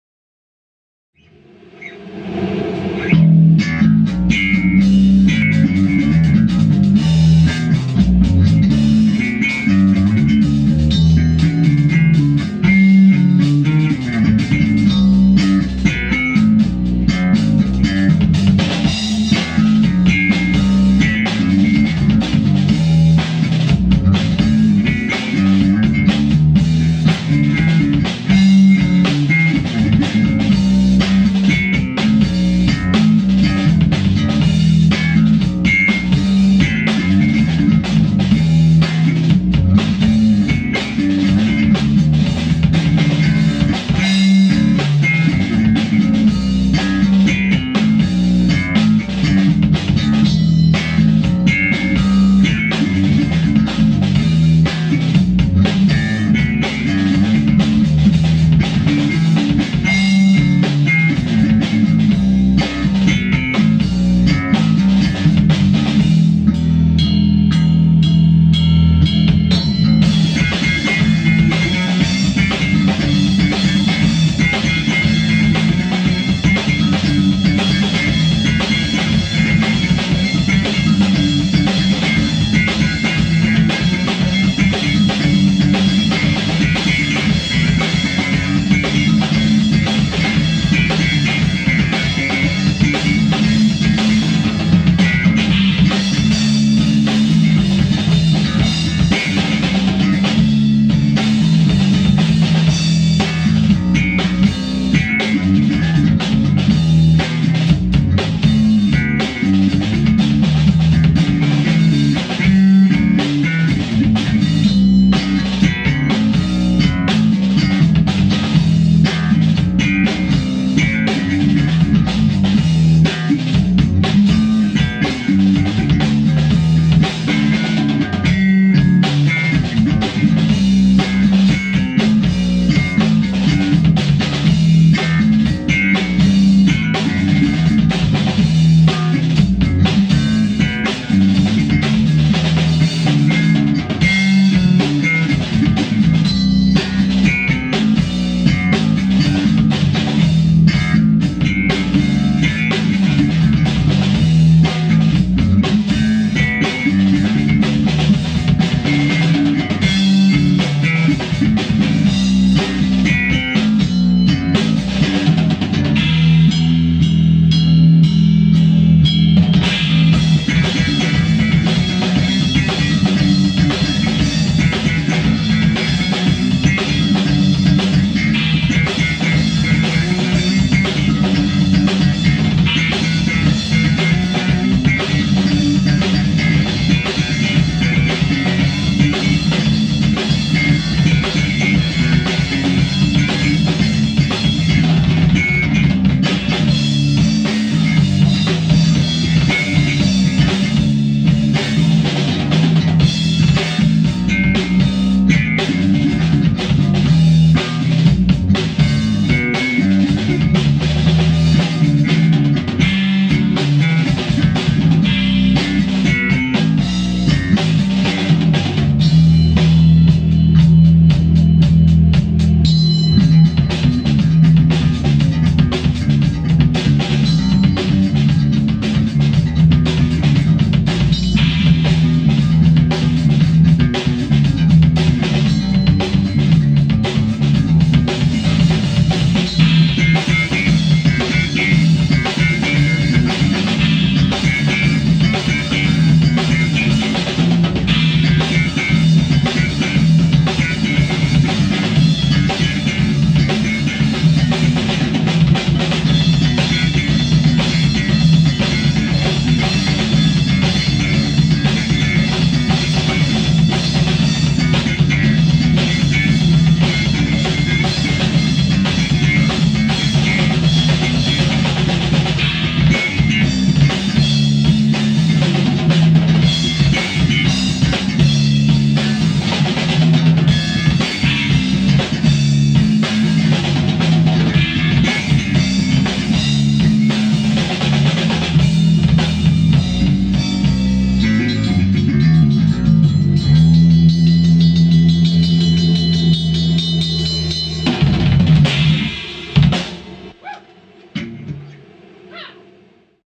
Bootleg Quality
A bootleg quality recording of an old practice session. Song was written and performed by Robin DeLecusto and Chaka Smash.